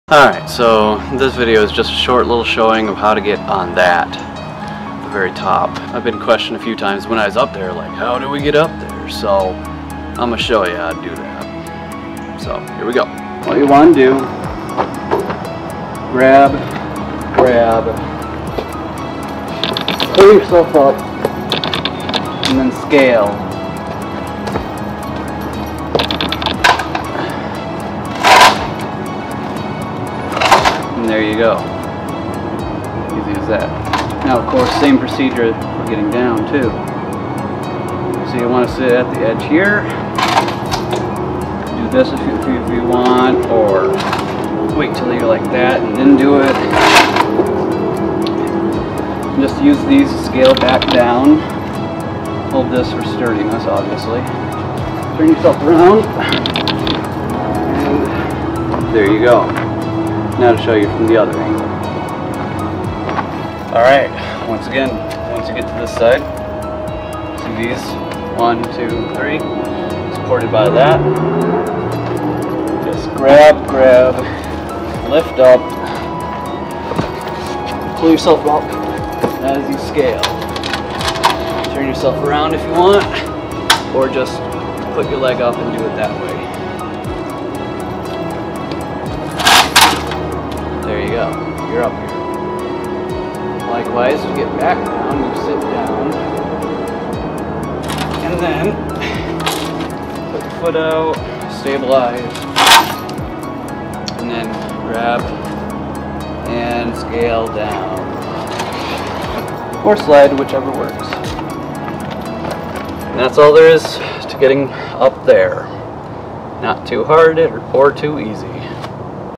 How To Get On Top Of The Circular Stone Pillar - Traverse City State Hospital - Hiking Trails -[4K]
Just a short demonstration or tutorial, whichever way you want to put it, on how to get on top of the circular stone structure that is at the top of the stairs in the Traverse City State Hospital Trails. I've been asked a few many times on how to get up there surprisingly, so I figure those doing their research prior to going on the trails, they could see this and know ahead of time and save them time on asking others as well. This was impromptu so it's not the best but you get the idea nonetheless.